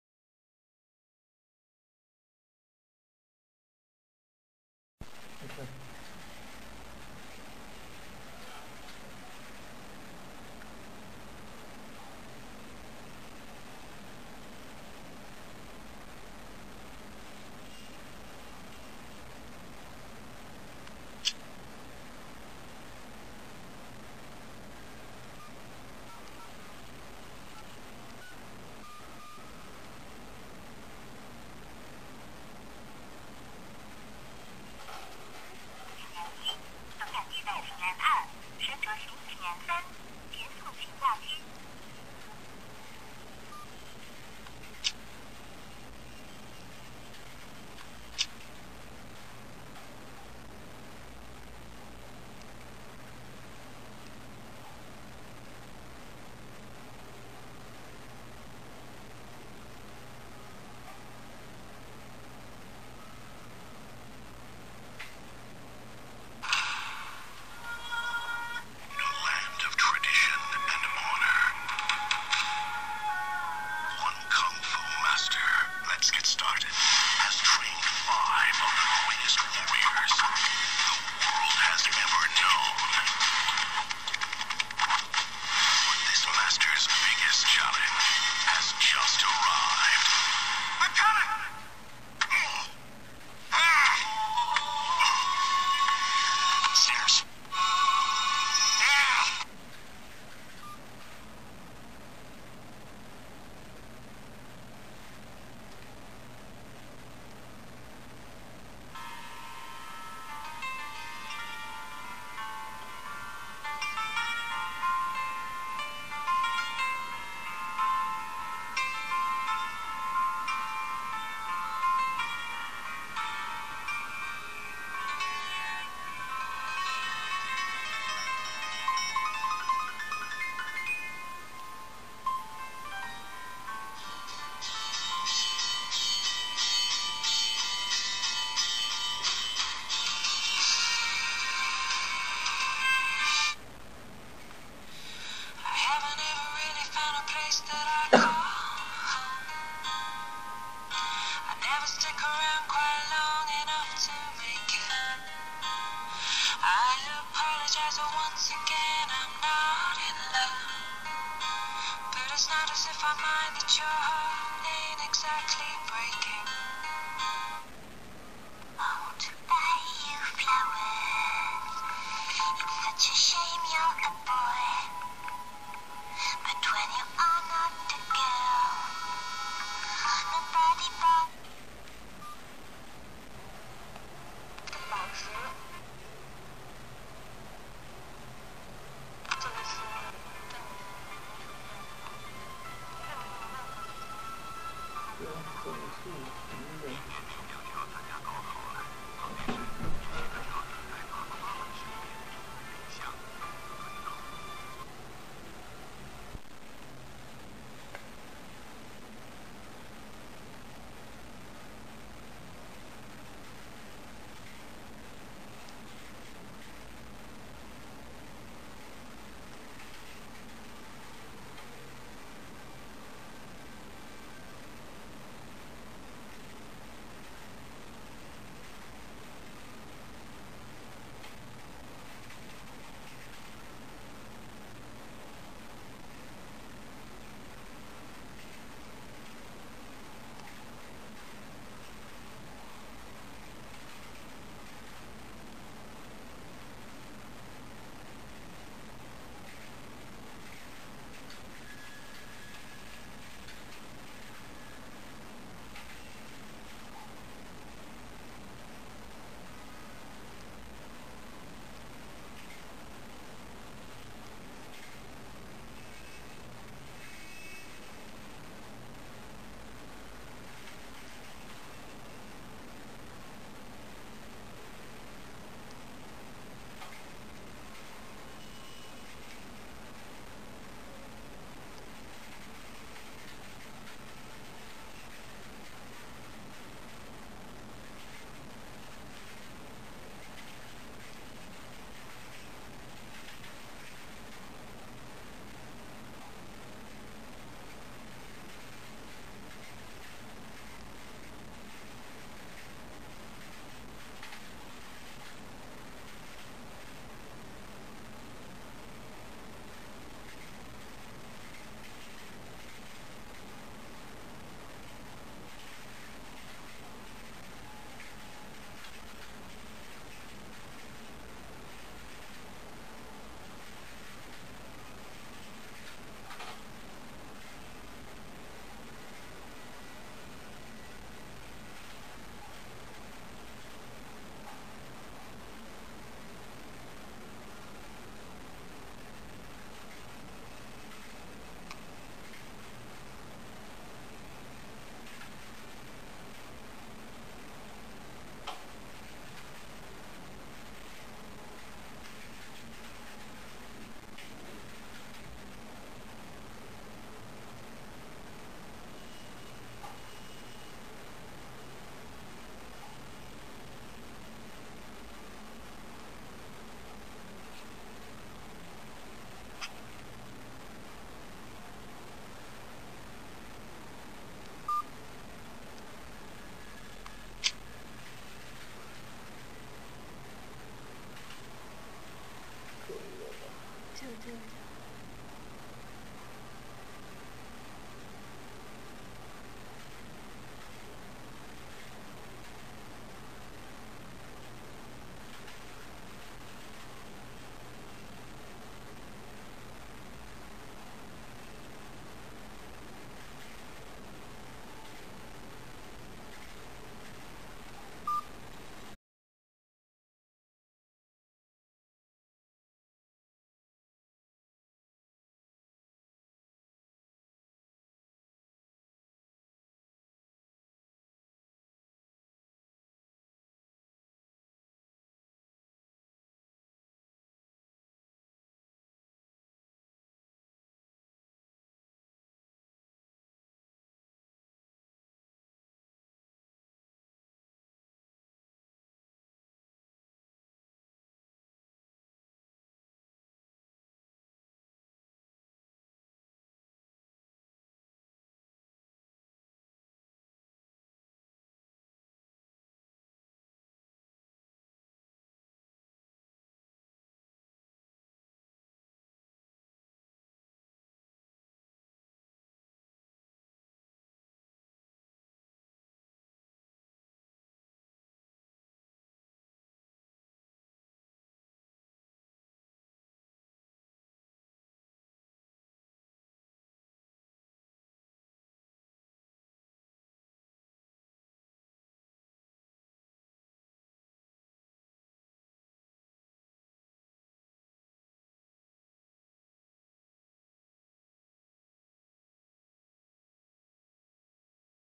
Hiphone i9 TV WiFi Dual SIM Standby Dual Cameras Unlocked Cell Phone with Java FM
Highlights
WiFi: 802.11b/g wireless internet
Quadband:GSM 850/900/1800/1900 MHz
3.2 inch touch screen; HVGA  PX:320*480
Dual SIM cards dual standby
Gravity inducer
JAVA 2.0
Handshaking Function:change wallpaper and music by shaking